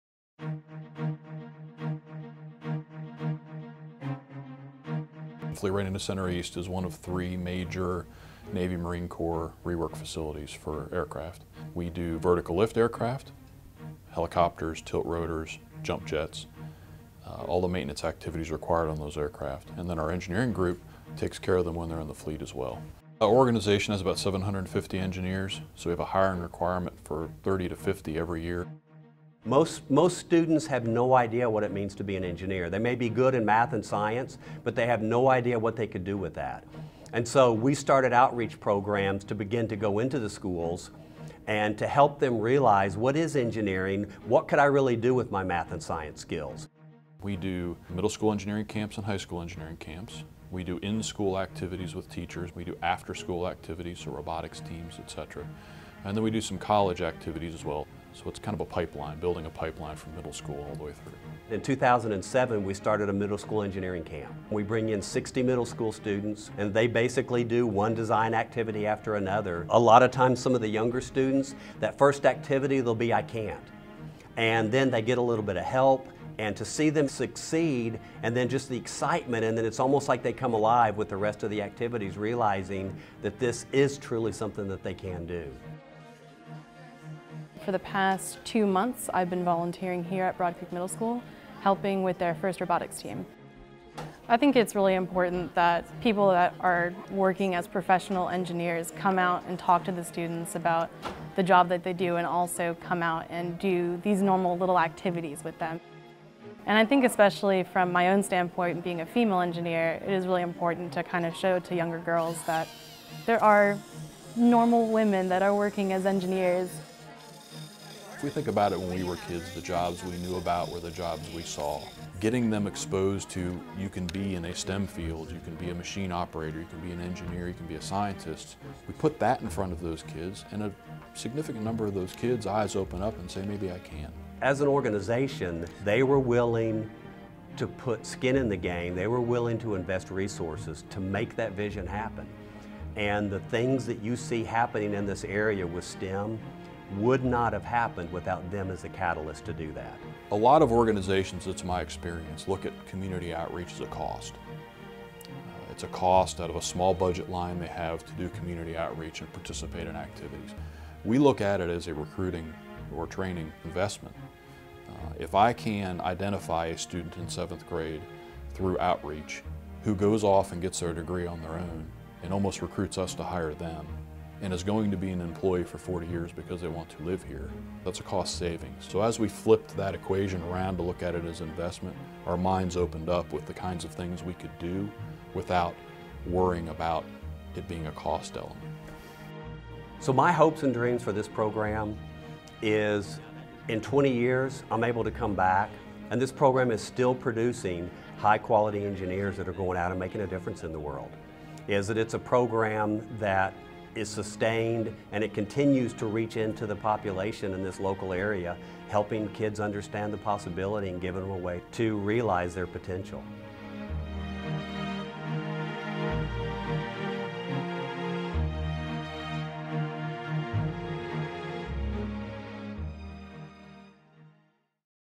2017 Business and Industry Award - FRC East
RESEARCH TRIANGLE PARK, N.C. – Cherry Point-based Fleet Readiness Center East (FRC East) has received the 2017 Outstanding Business and Industry Award from the N.C. Science, Mathematics, and Technology Education (SMT) Center.  FRC East was recognized at the annual SMT celebration on April 29 in Raleigh, N.C.

This award recognizes a North Carolina business or industry whose support has strengthened science, mathematics, and technology education programs in either formal or informal settings.

“Fleet Readiness Center East is a leader in STEM initiatives in eastern North Carolina,” said Dr. Sam Houston, CEO and president of the SMT Center.  “Fleet Readiness is a model for other businesses attempting to partner with the education sector.”

Fleet Readiness Center East began operations during World War II at MCAS Cherry Point, N.C.  Its initial workload consisted of several well-known aircraft, including the Corsair fighter and the Mitchell B-25 and Marauder B-26 bombers.   The depot continues to provide maintenance, engineering and logistics support on a variety of aircraft, engines and components for all branches of the U.S. armed forces.

Today, FRC East is a modern industrial complex that has considerable impact on the economy of North Carolina and the communities surrounding the air station. With annual payroll that exceeds $275 million, the facility is North Carolina's largest industrial employer east of Interstate 95.

For more than 10 years the company has developed regional collaborative programs in STEM education to support project-based learning with a focus on aerospace engineering.  FRC East also sponsors a mobile fabrications laboratory for students and educators use.

The N.C. Science, Mathematics, and Technology (SMT) Center is focused on improving education as a means of providing all students in North Carolina with the knowledge and skills to have successful careers, be good citizens, and advance the economy of the state. Promoting innovation and change in education, the SMT Center celebrates educational excellence in North Carolina by supporting educators, engaging students, and advancing the growing importance of having a STEM-educated workforce.